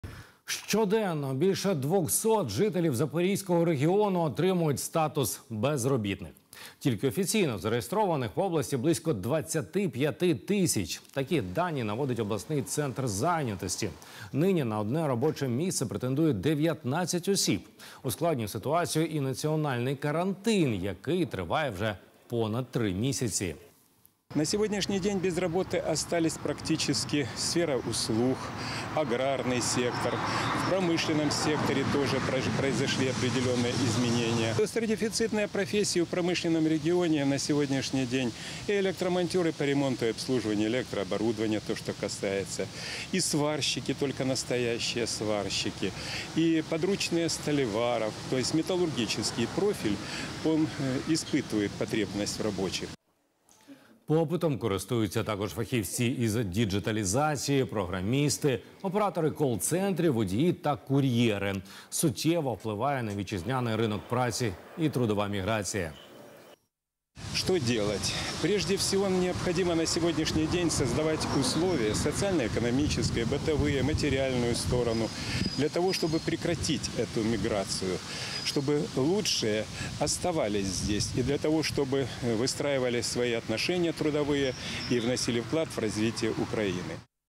Проблема безробіття у Запорізькому регіоні
Щоденно більше 200 жителів Запорізького регіону отримують статус безробітних. Тільки офіційно зареєстрованих таких громадян по області близько 25 тисяч. Такі дані наводить обласний центр зайнятості. Нині на одне робоче місце претендує 19 людей. Ускладнює ситуацію й національний карантин, який триває вже понад три місяці.